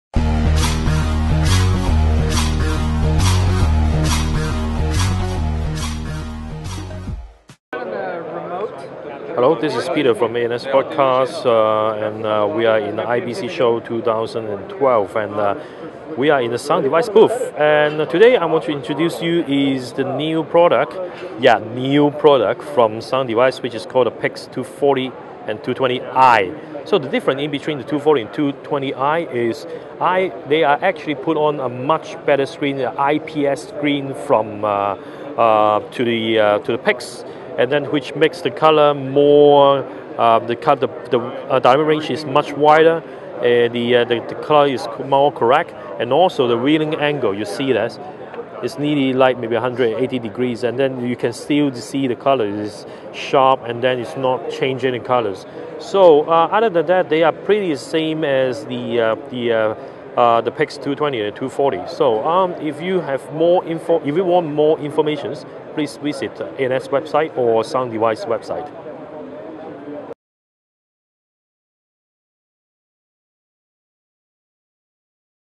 IBC 2012 Live Review - Difference between Sound Device PIX240 and PIX240i
This video introduces the difference between PIX-240 and PIX-240i.